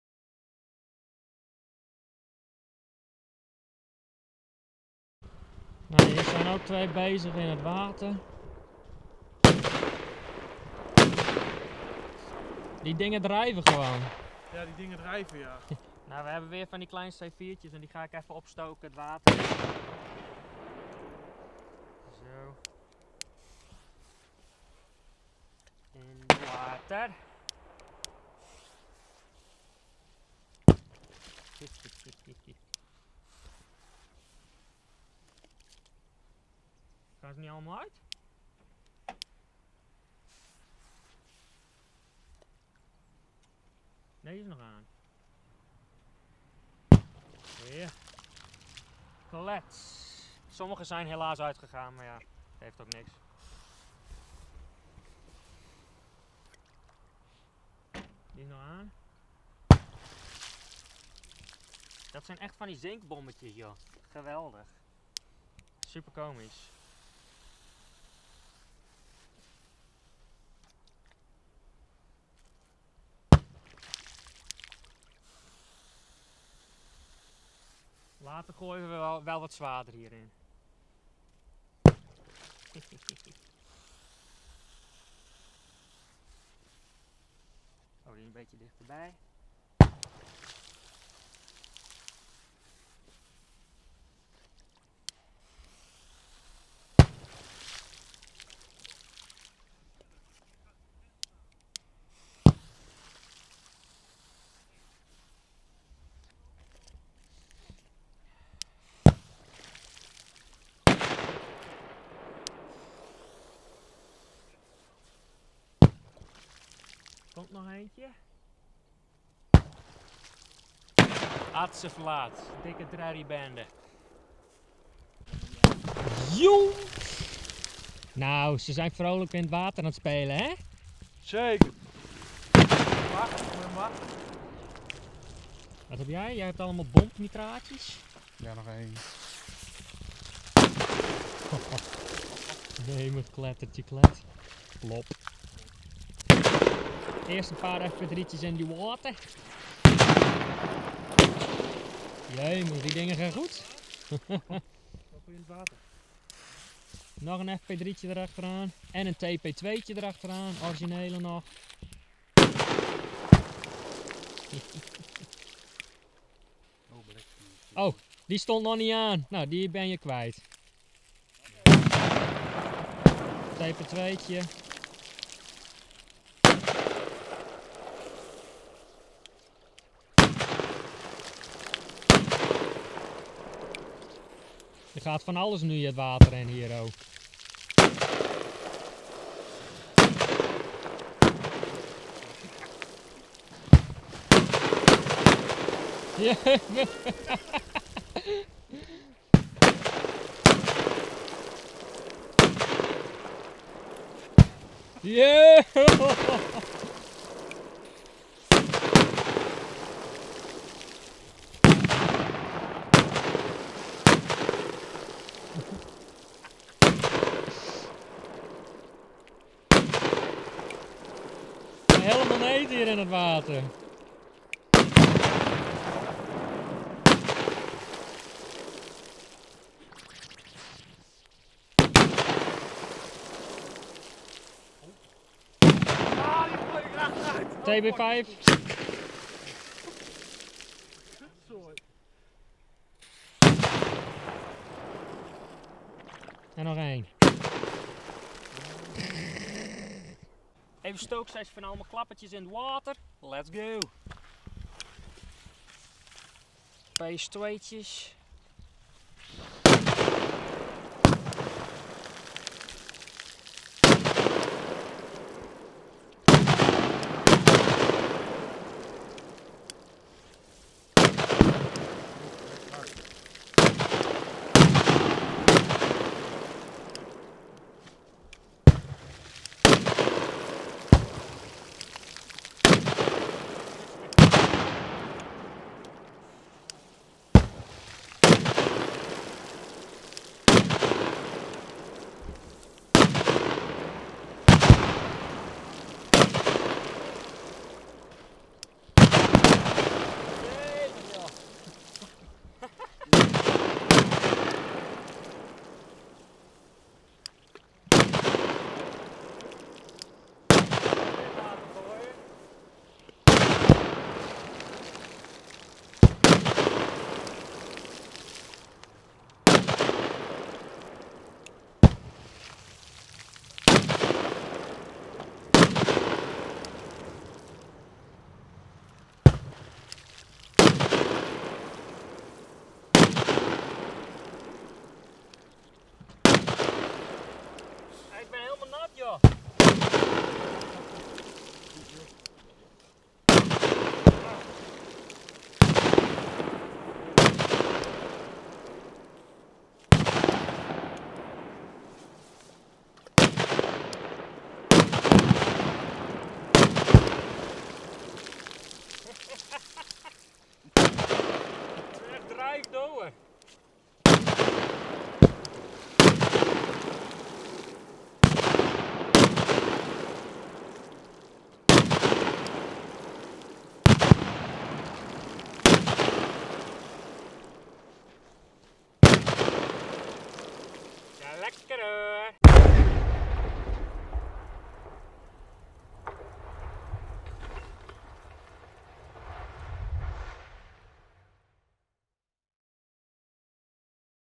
Nitraten in het water
NL: In deze video gooien we verschillende knallers in het water wat altijd een erg leuk effect geeft

Warning! Alles wat we deden was in polen! Alles wat we in polen deden was volgens de Poolse wet volledig legaal!! Maar alsnog raad ik niemand aan om dit na te doen! Ik ben niet verantwoordelijk voor jou acties!
ENG: In this video we throw various firecrackers into the water, which always gives a very nice effect

Warning! Everything we did was in Poland! Everything we did in Poland was completely legal under Polish law!! But I still don't recommend anyone to do this! I am not responsible for your actions!!

and do you want to help me so i can make more awesome videos? you can always donate with credit card and paypal